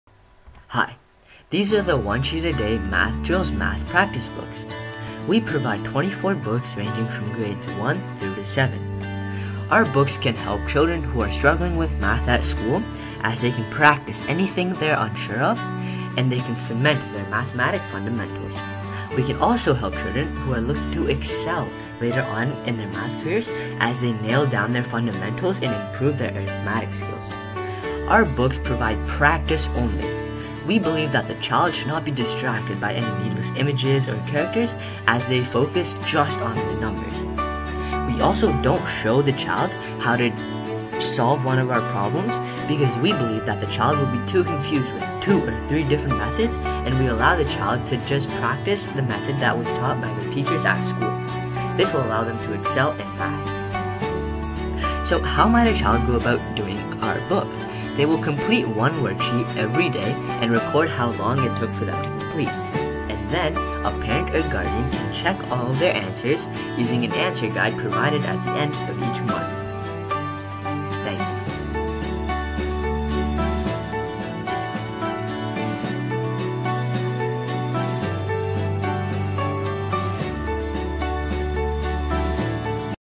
One-Sheet-A-Day Math Drills, 24 volumes for grades 1 through 7
Upon completion, the student will be more competent in 1st grade mathematics and ready to proceed with more difficult exercises. Unlike other math drill books, this is one of 24 workbooks created to establish a daily routine for each student to practice on their own from grades 1 through 7. It is specifically designed to gradually increase mathematic ability with the least amount of stress for both parent and student.

Kids need to practice at home to support what they are learning in school, but finding resources to help with that goal can be daunting. It takes a lot of time searching online for free resources to print or for books with enough exercises. Beyond that, the options include going to libraries, enrolling them in an afterschool class or hiring a personal tutor. Who has the time and money for all of that? The answer is, not many parents.

Each and every parent wants to provide enough practice work to their kids, but on top of the challenges in finding resources, questions remain about how much practice a child needs to boost educational success: twenty sheets of practice work a day? One sheet a week? What’s the optimum practice work during a whole academic year? Confused?

The authors have a decade of classroom expertise and have spent a large amount of time researching and wasting resources trying all the options for their own kids. Now you can benefit from what they have learnt. The result is finding out that “ONE-SHEET-A-DAY” is the optimum way to support classroom learning, and it is amazingly simple.

This book eliminates the need to take home prints or run around franchises, bookstores and libraries trying to find enough material. It also prevents you from overwhelming your kids with too many math drill worksheets. There are approximately 200 school days a year. Simply have your child complete ONE per day.